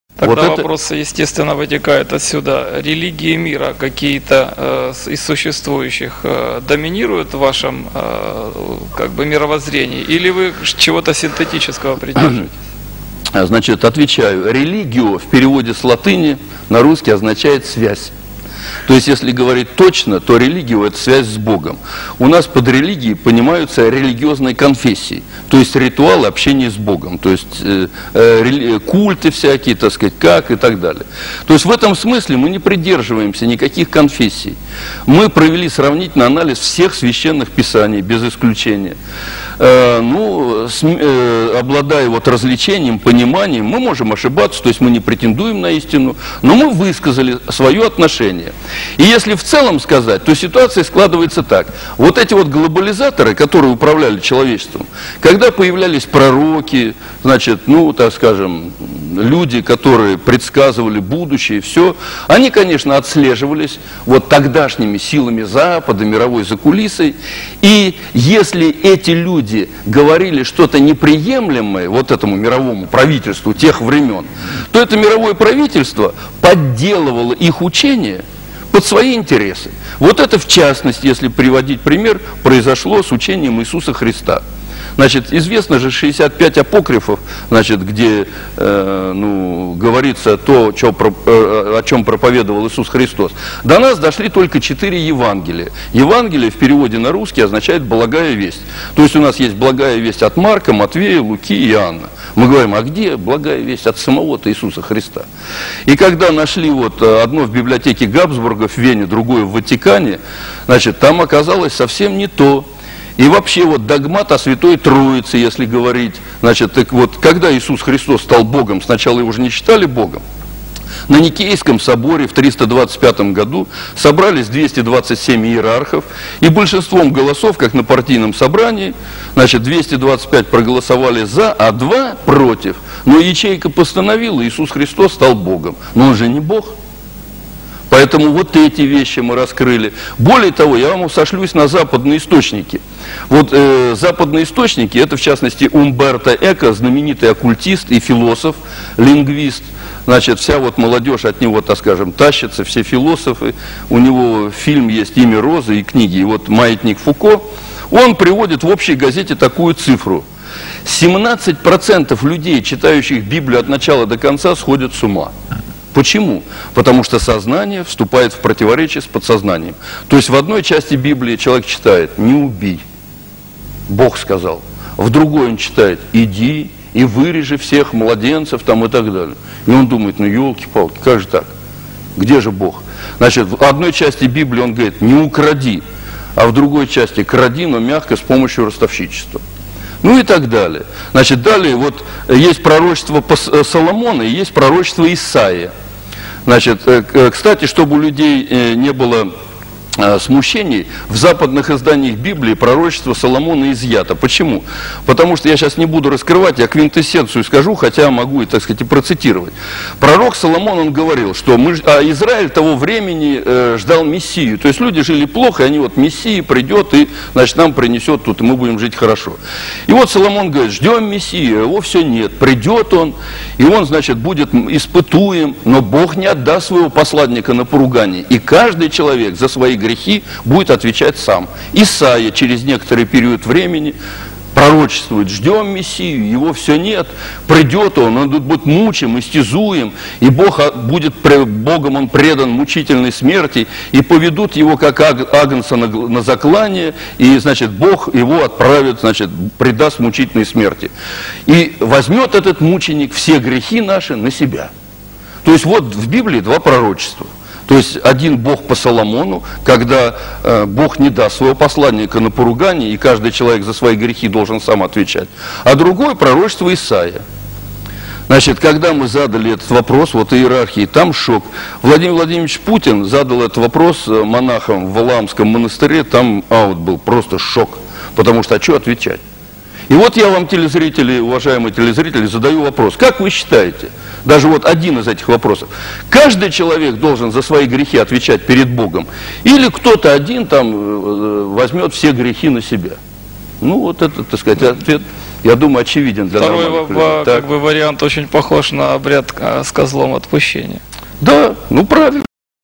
Нестыковки в библии (или точнее косяки), генерал Петров
любителям бездумно восхвалять эту книгу ( "библия" переводится как "книга")